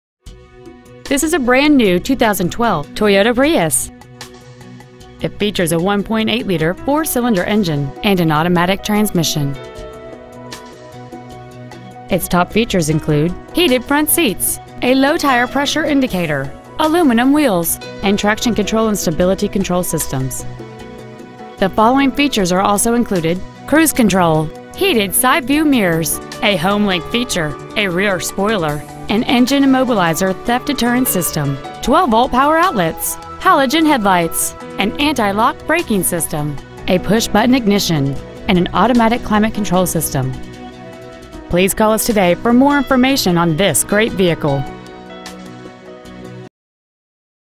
2012 Toyota Prius Grapevine TX 76051
Year : 2012
 Make : Toyota
 Model : Prius
 Engine : 1.8L DOHC 16-valve VVT-i Atkinson-cycle I4 hybrid engine
 Trans . : Automatic
 Exterior : Silver
 Miles : 3
 Interior : -

 801 State Hwy. 114 East